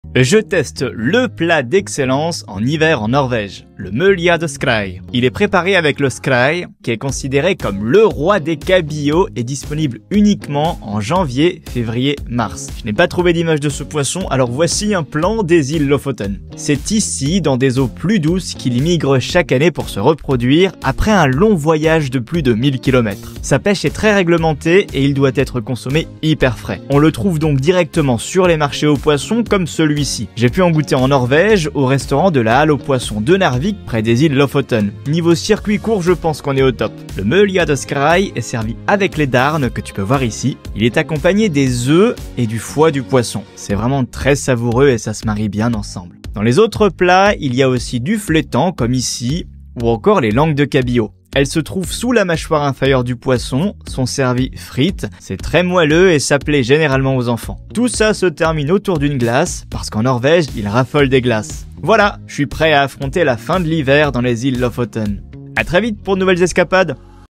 🐠 Le Mølje de Skrei
🐠 La Norvège, c’est le paradis pour les fans de poisson ! J’ai pris un Mølje de Skrei, avant d’apprendre que c’était LE plat emblématique de la région, dispo à cette période très précise de l’année. Un régal !
🗣️Je teste le plat d’excellence en hiver en Norvège : le Mølje de Skrei

Il est préparé avec le Skrei, qui est considéré comme le roi des cabillauds et disponible uniquement en janvier-février-mars. Je n’ai pas trouvé d’image de ce poisson, alors voici un plan des îles Lofoten.

C’est ici, dans des eaux plus douces, qu’il migre chaque année pour se reproduire, après un long voyage de plus de 1000km.

Sa pêche est très règlementée et il doit être consommé hyper frais. On le trouve donc directement dans les marchés aux poissons, comme celui-ci.

J’ai pu en goûter en Norvège, au restaurant de la halle aux poissons de Narvik près des îles Lofoten. Niveau circuit court je pense qu’on est au top.

Le Mølje de Skrei est servi avec les darnes que tu peux voir ici, il est accompagné des œufs et du foie du poisson. C’est vraiment très savoureux et ça se marie bien ensemble.

Dans les autres plats il y a aussi du flétan comme ici, ou encore les langues de cabillaud. Elles se trouvent sous la mâchoire inférieure du poisson, sont servies frites, c’est très moelleux et ça plait généralement aux enfants !

Tout ça se termine autour d’une glace, parce qu’en Norvège ils raffolent de glaces 🙂

Voilà, je suis prêt à affronter la fin de l’hiver dans les îles Lofoten !

À très vite pour de nouvelles escapades